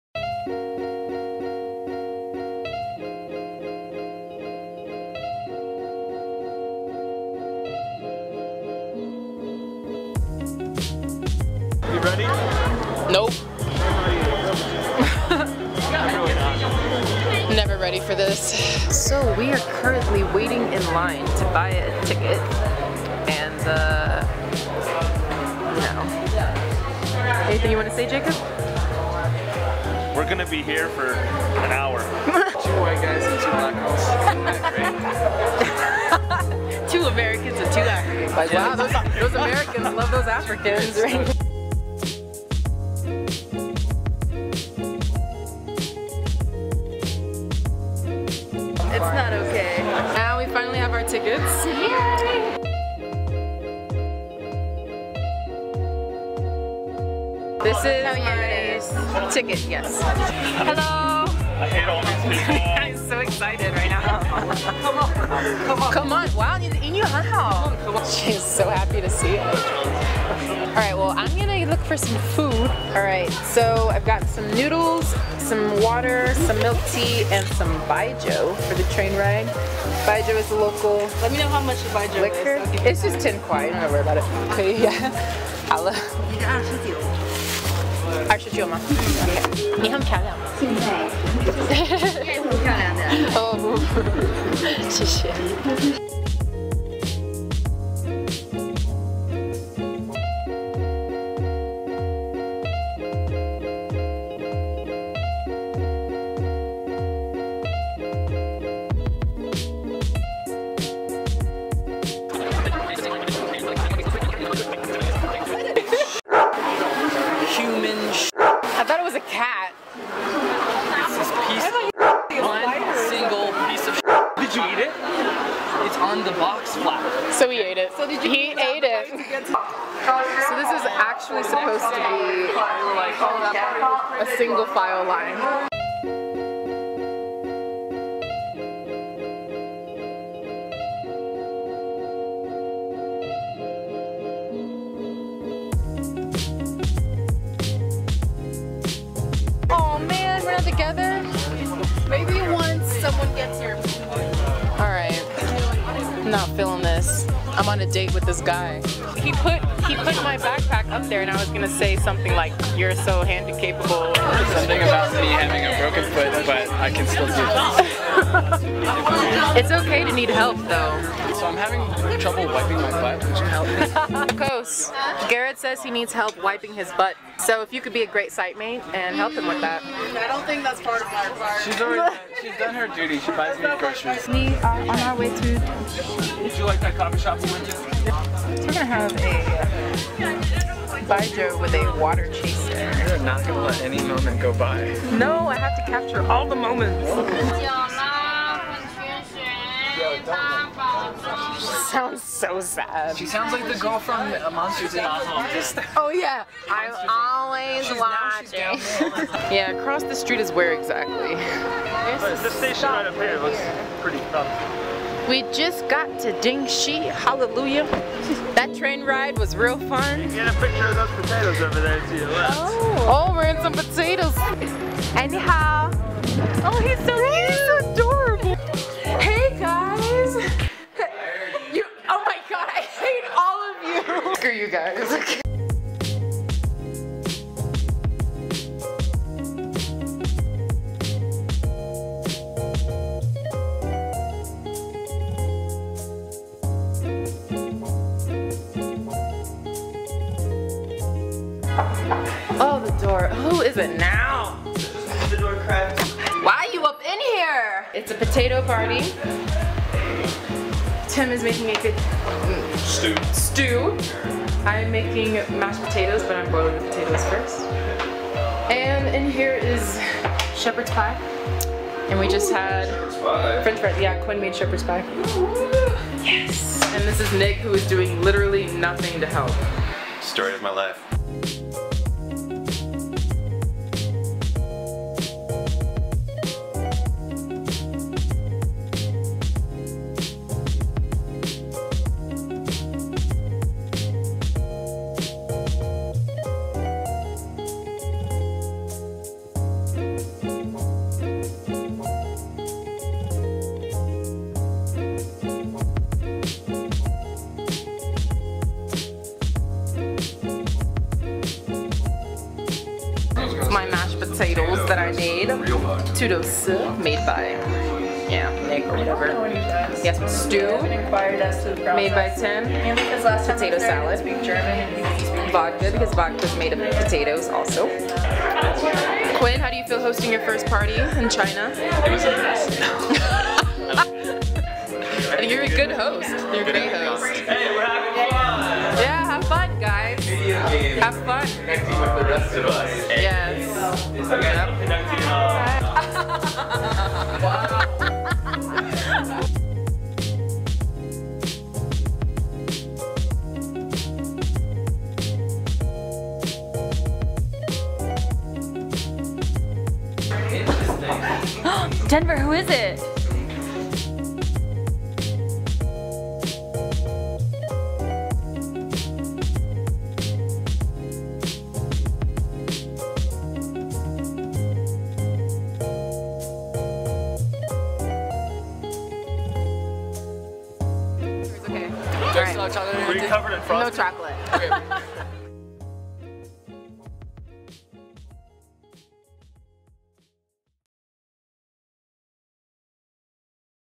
Peace Corps China: Gansu's 1st Annual Potato Party!
Check us out as we venture into Dingqi to Visit Quinn and Jaymey for a night.